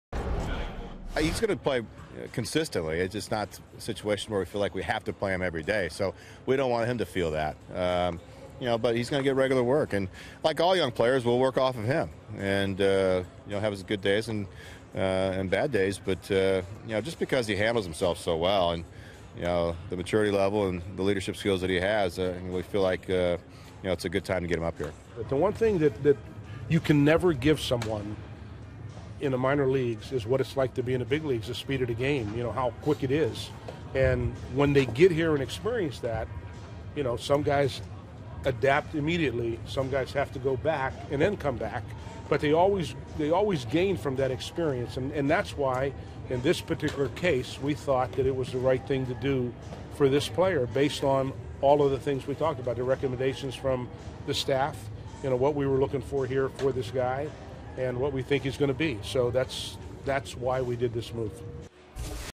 2013/06/12 Wedge, Zduriencik discuss Zunino
HOU@SEA: Wedge, Zduriencik talk call up of Zunino